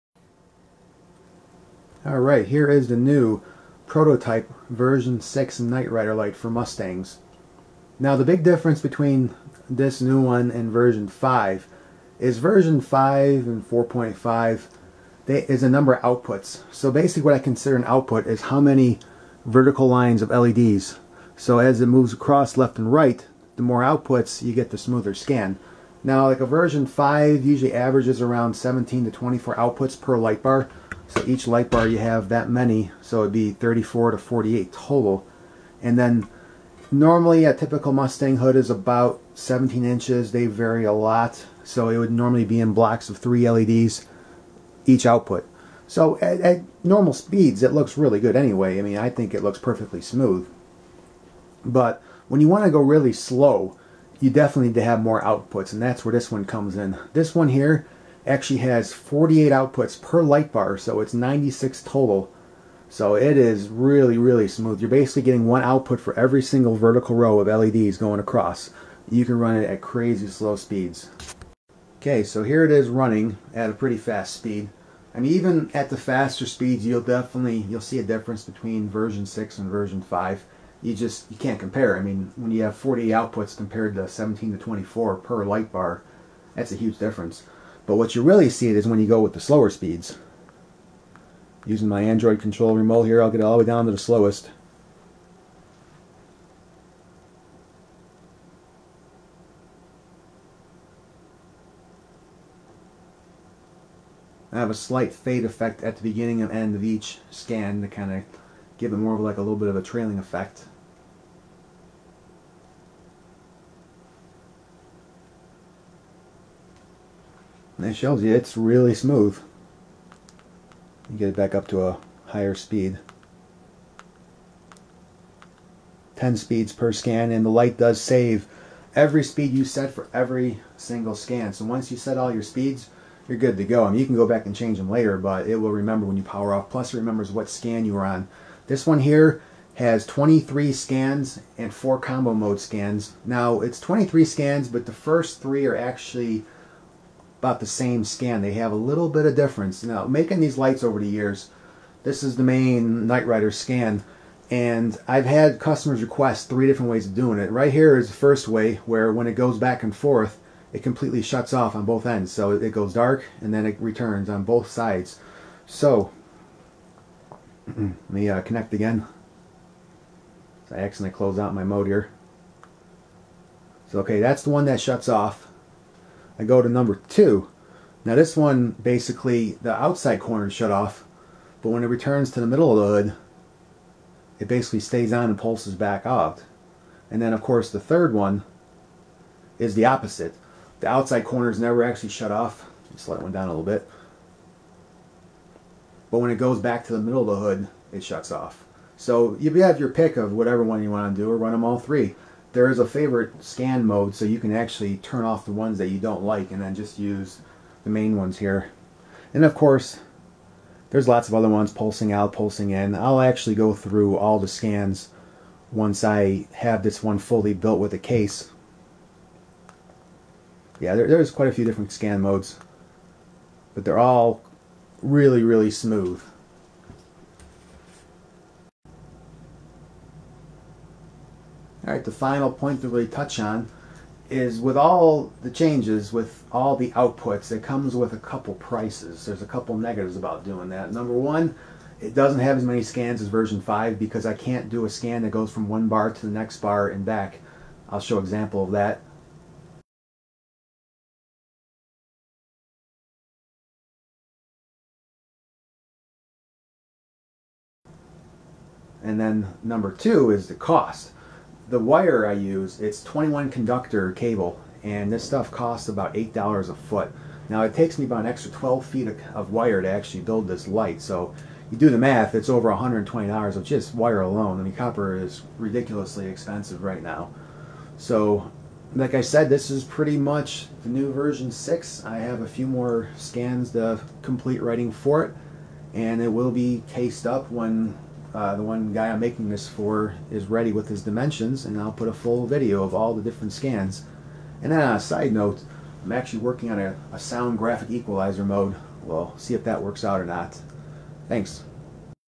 DISCONTINUED Mustang Knight Rider Light Scanner Version 6
Update:   I ended up discontinuing this version.  It took way too long to build and uses up too much of my special cable which is unavailable to purchase anymore.  There are a few other issues as well that I don't have the time to work out.    Version 5 is still the best to way to go for Mustangs.

Here's the prototype of my new Version 6 Mustang Knight Rider light.  The main difference on this light over Version 5 is the number of outputs.  My version 5 ranges from 17-24 outputs per light bar (34-48 total) where this new version 6 has 48 per light bar (96 total).  The smoothness of the scans are incredible especially at very slow scan speeds.  It has 24 scans with 10 speeds per scan and is remote control and has Android control as well.   The LEDs I use are the same ones from version 5 so the brightness is the same.

The idea to build this scanner just hit me one day.  Personally I think Version 5 is a great scanner.  I love the scoop to scoop scans from one to the other which this one lacks but if you want to have the smoothest light around, this is it.    The other downside is the price.  I need 12ft extra of my 21 conductor cable to build this light and it costs $8 a foot.  That doesn't include the extra 2 hours it takes me to wire it up.